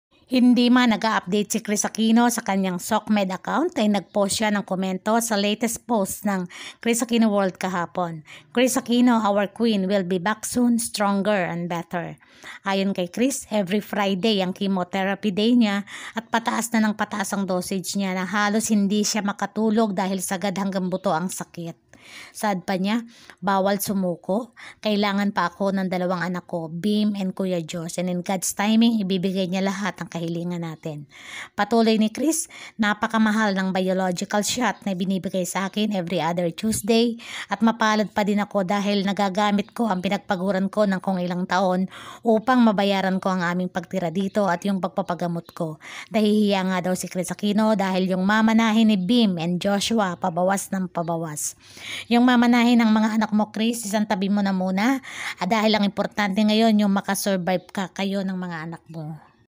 KRISAQUINO WILL BE BACK SOON, STRONGER & BETTER! MAMANAHIN NINA BIMB & JOSH BAWAS NA!
Hindi man nag a-update ang queenofallmedia krisaquino ay nagpost naman sya ng komento sa latest post krisaquinoworld kahapon: Kris Aquino, our Queen will be back soon! Stronger & Better!
Ayon kay kris, every friday ang chemotherapy nya at pataas na ng pataas ang dosage na halos hindi sya makatulog dahil sagad hanggang buto ang sakit.
Saad pa nya, bawal sumuko, kailangan pa ako ng dalawang anak ko, Bimb & Josh..and in God's timing, ibibigay NYA ang kahilingan natin..
Patuloy ni krisaquino, napakamahal ng "biological shot" na ibinibigay sa akin every other tuesday. Mapalad pa din ako dahil ung pinagpaguran ko ng ilang taon, ay nagagamit ko upang mabayaran ung pagtira namin dito at ung pagpapagamot ko.. nahihiya nga daw itong si kris dahil ung mamanahin nina Bimb & Josh bawas na!
Kris, eto lang masasabi ko,  kung mamanahin ng mga anak mo, isantabi mo na muna, saka mo na isipin un, dahil mas ang mas mahalaga ngayon, ung kagali gan mo, ung makasurvive ka, kayo ng mga anak mo...
Ako nga, hindi ako nag aalala kung saan na kami titira bukas or may ipakakain pa ba ako sa mga anak ko, dahil alam kong may Dios na SYANG mangangasiwa ng lahat! God is good provider! Sabi mo nga, ipagkatiwala natin sa Kanya ang lahat!
We'll continue praying for you, dahil naniniwala ako, walang imposible sa Kanya!🙏